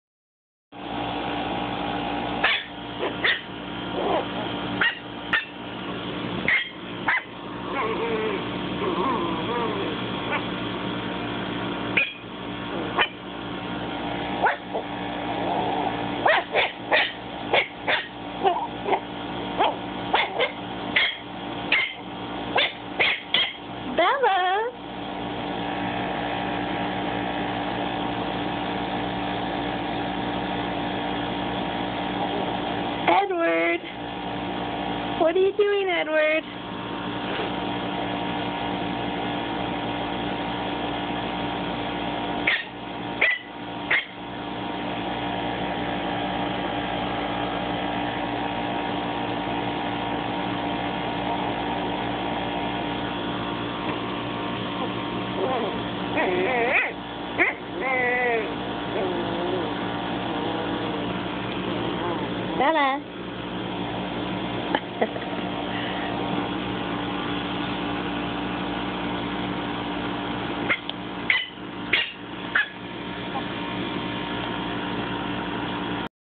Maltipoo and cat
My Maltipoo Bella playing with my cat Edward.